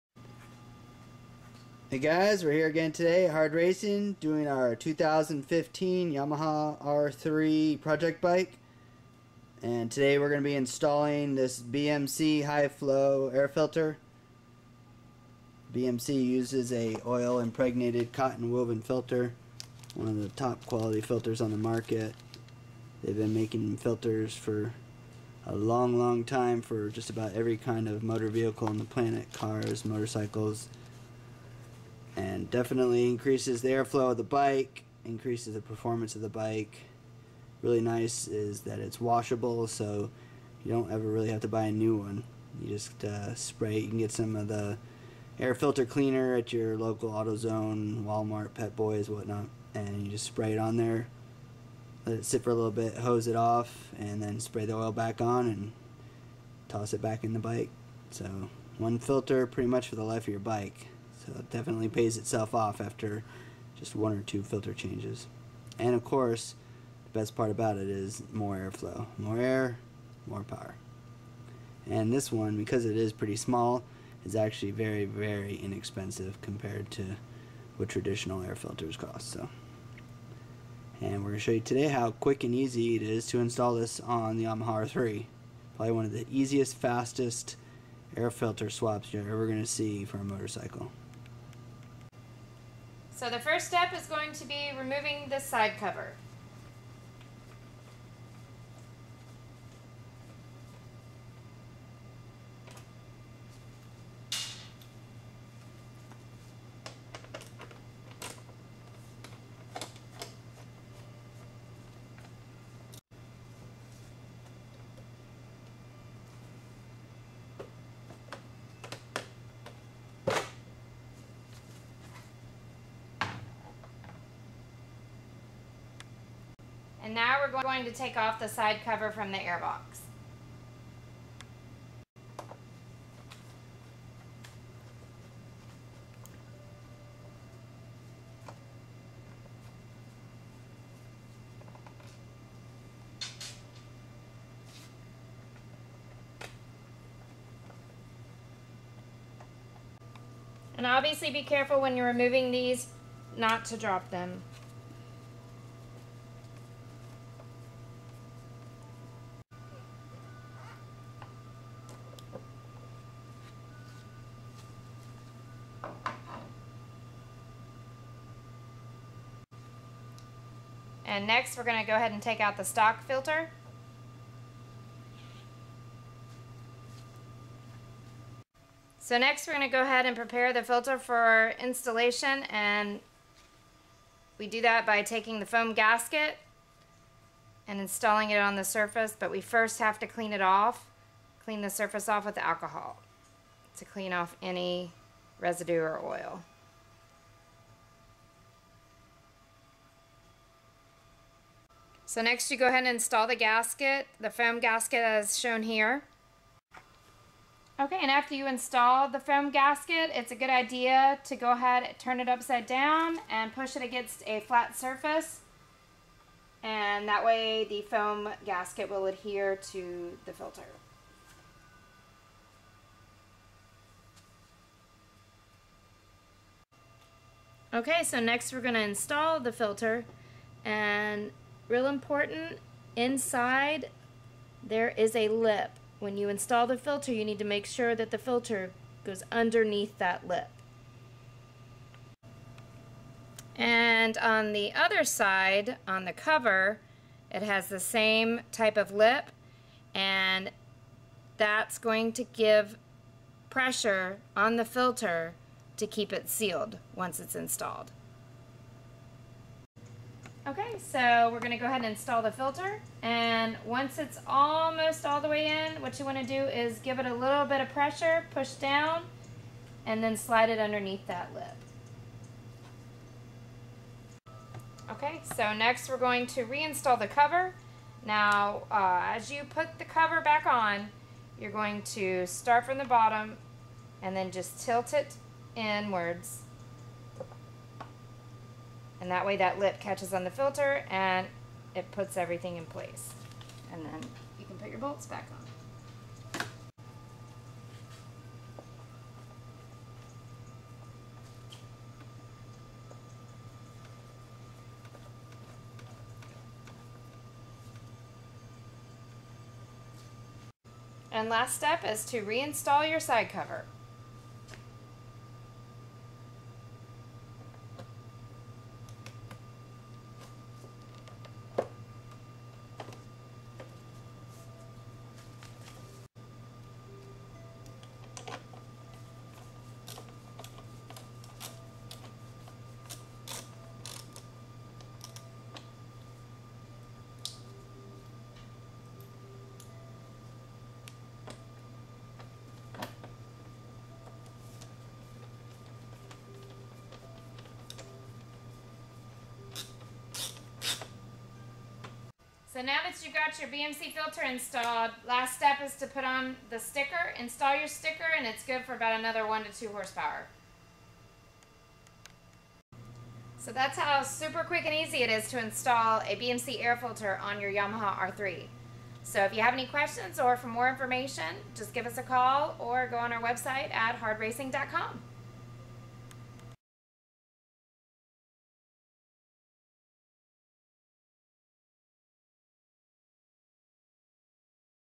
2015 Yamaha R3 BMC Air Filter installation - Very Quick and easy to do
Installing BMC Air Filter on a Yamaha R3. This is one of the quickest and easiest upgrades you can do for your bike.  Only takes a few minutes.   Increase the Airflow, add a little power.  Completely washable, so you almost never have to replace it.

→ Please take a second to leave a comment before you head off.